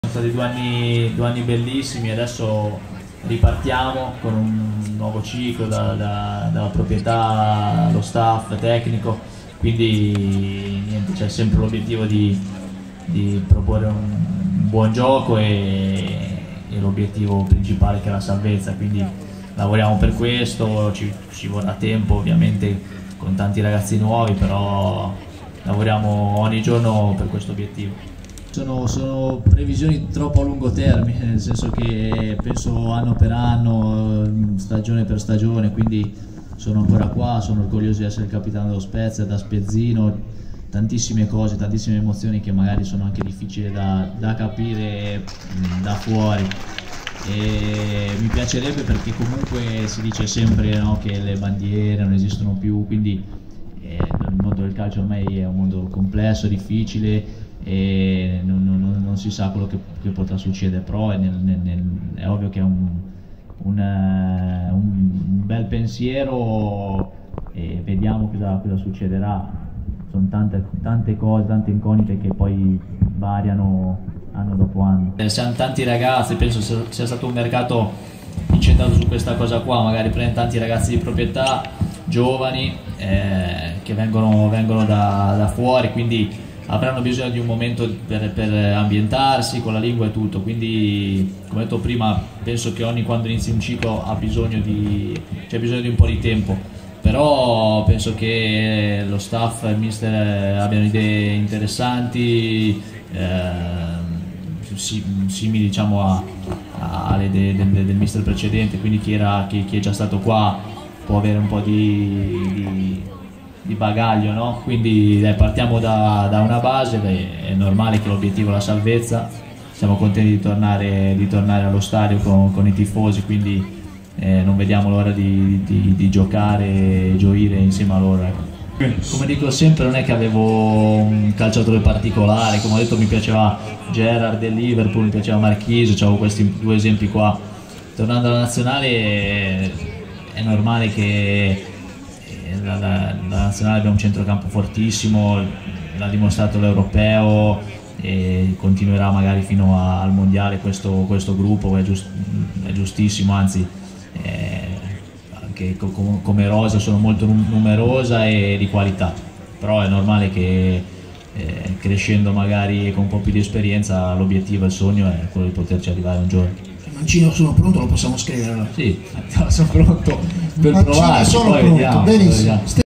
Giulio Maggiore riceve il premio Scarabello e parla dello Spezia, del campionato e della Nazionale
Giulio Maggiore ha ricevuto l'ambito premio Scarabello nella serata di Aulla. Nelle parole del capitano aquilotto ci sono il campionato che si sta giocando, la voglia di essere un simbolo della squadra e della città e il sogno della Nazionale